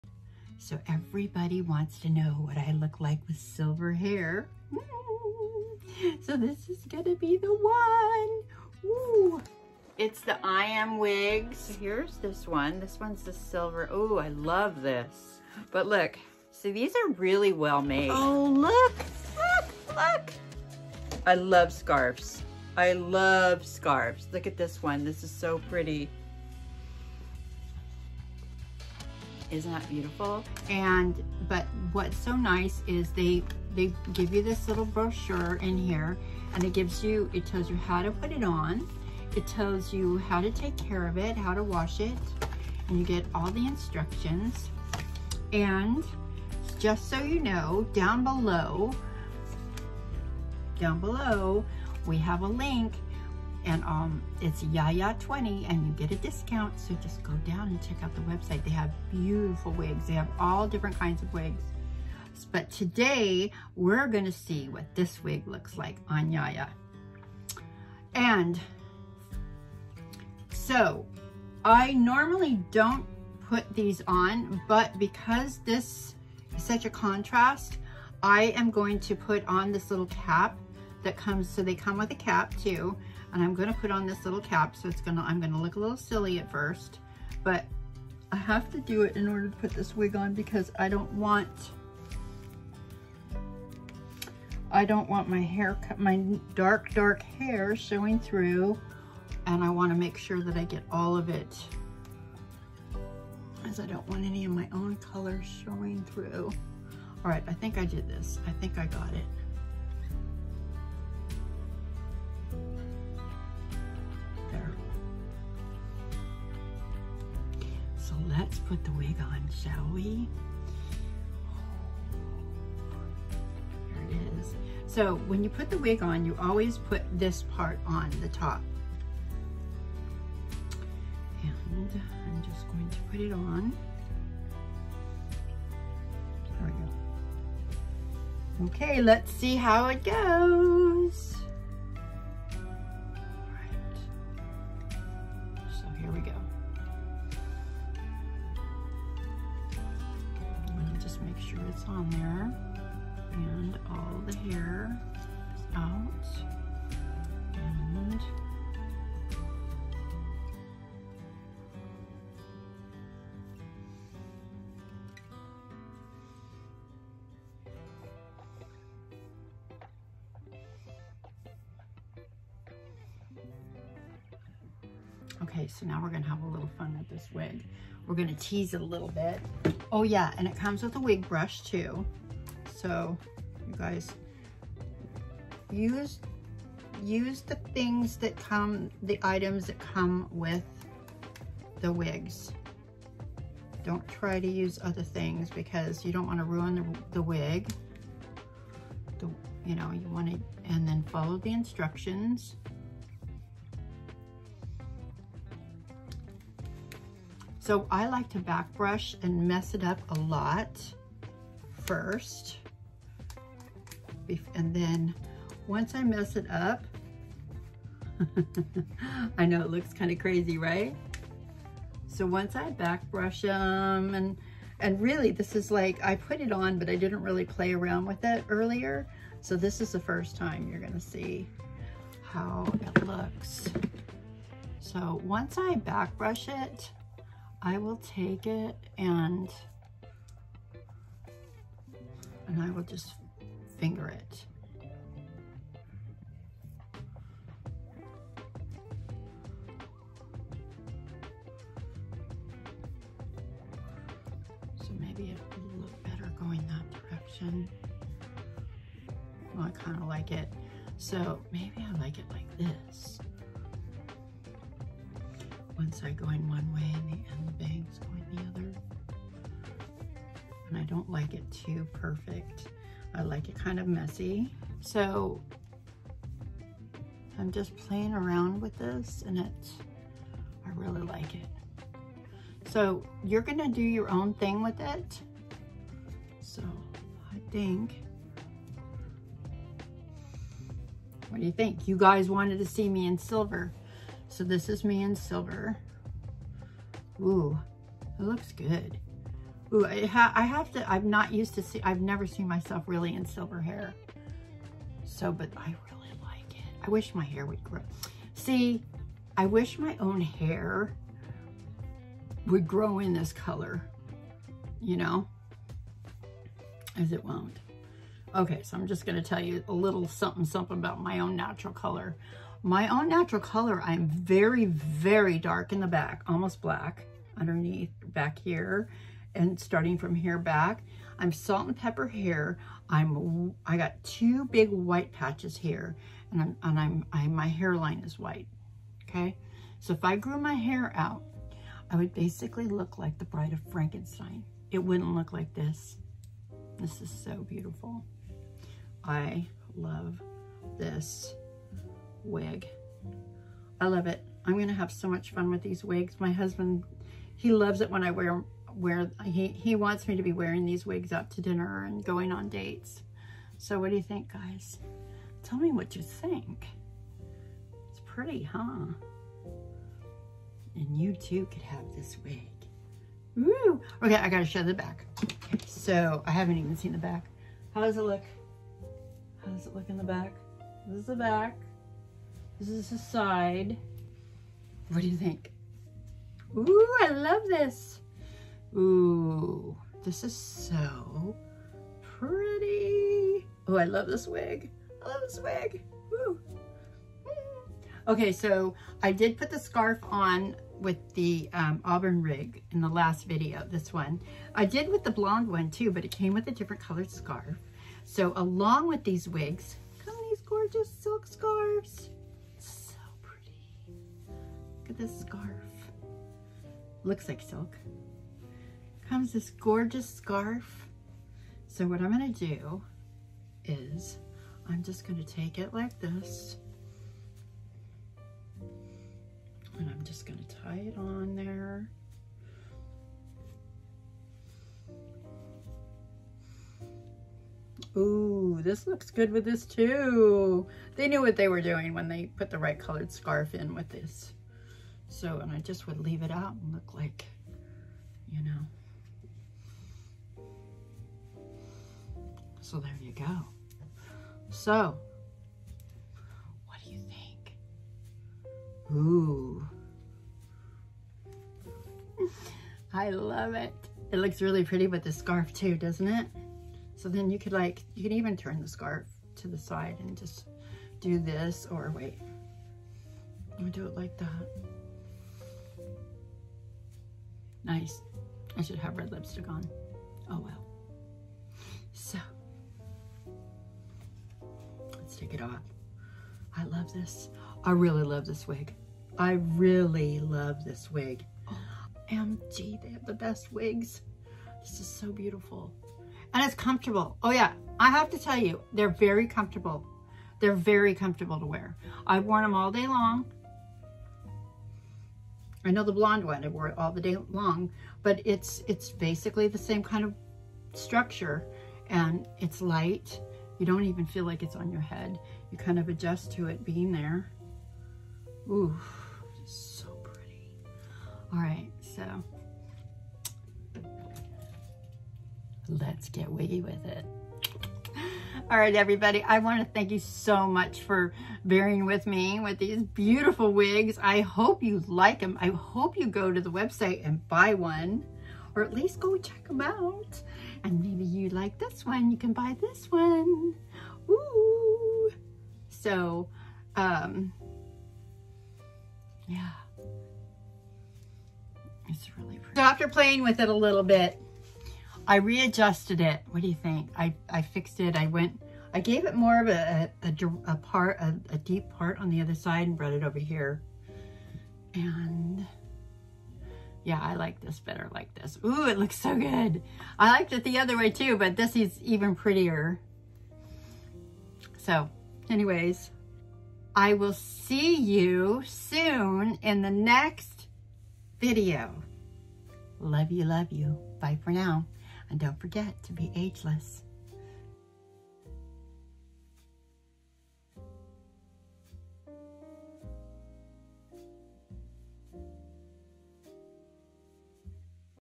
Over 60 hair fashion! The Silver Wig! Let’s get Wiggy With it!
Today, I am putting on the silver wig that I received last week. Remember a previous video I put on an auburn wig for you, and about six weeks ago I put I did a review on a blond wig, which was so fun a links are below for both of them. This was another fun video to make. I hope you enjoy it.